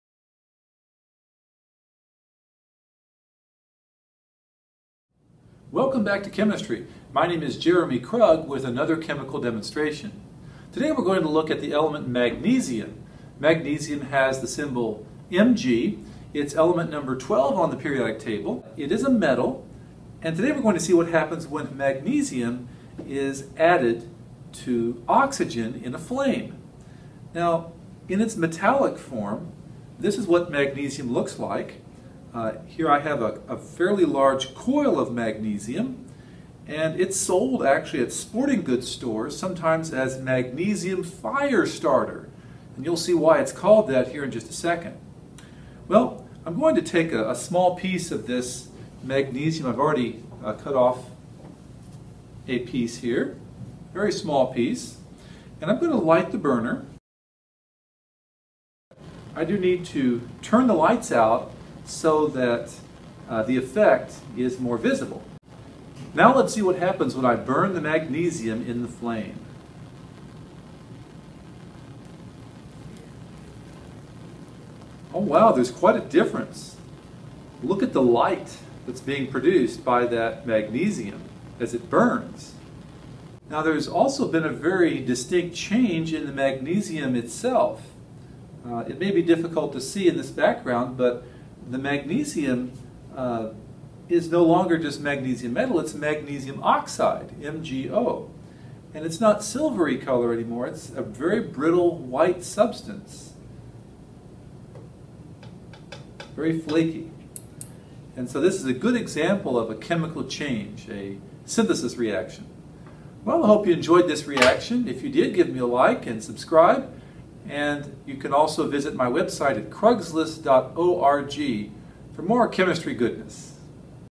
Burning Magnesium
In this video, Mr. Krug burns a small piece of magnesium, producing magnesium oxide, heat, and a very bright light!